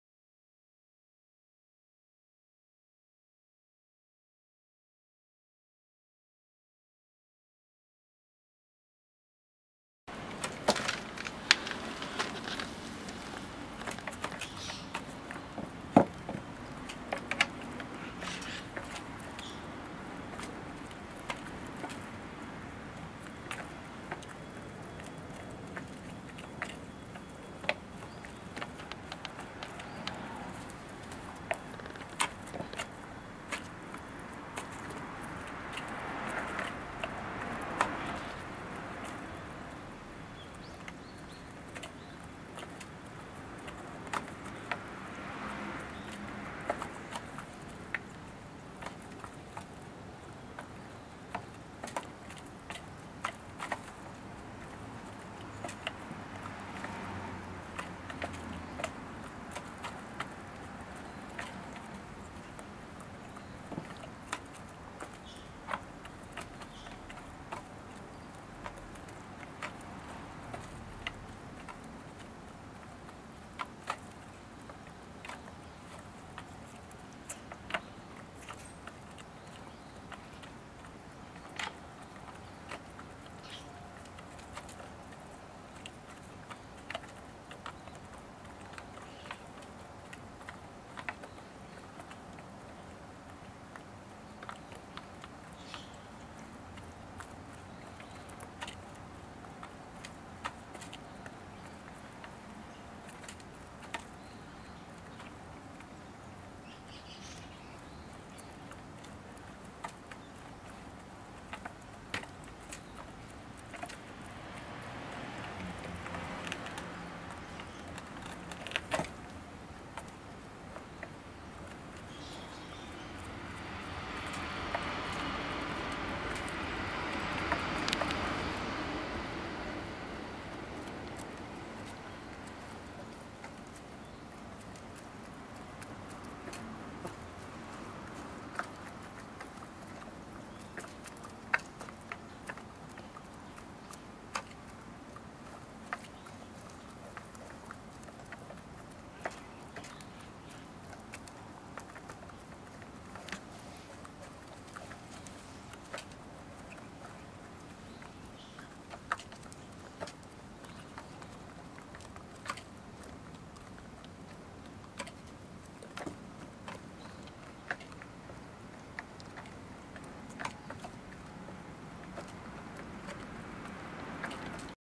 名古屋コーチン男子と父鶏の食事～にわとりの序列・マナーで優しく叱られる～
8か月の若鶏“夏”は、ときどき、年上コッコから叱られます。
ニワトリ社会では、序列がハッキリしていて、年上やボス（雄鶏）に逆らってはいけません。
食事のマナーは、雌鶏さんの序列上位から好きな場所に陣取りご飯を食べます。
ボス（雄鶏）は、序列が一番高いけど、優しいから、雌鶏さんに譲って遠慮することが多いですよ。

ぴよ助父さんは、息子の夏には甘く、雌鶏さんの方が夏に厳しく接することが多いです。
今回のビデオは、食事の時に「夏は父さんの後に食べなさい」と諭す場面の映像です。
クチバシをツンとやっているだけだから、夏本人は叱られている自覚が余りありませんね（笑）。